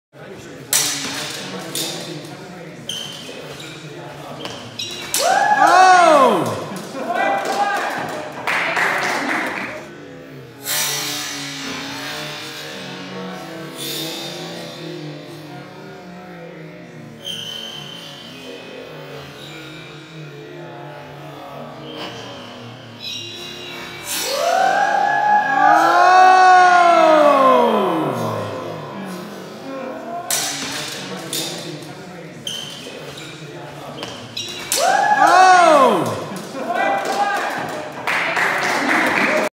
Ringen am Schwert
Quick clip from our monthly sparring night February 23, 2018. Novice Ben successfully applies a wrestling at the sword against instructor Chris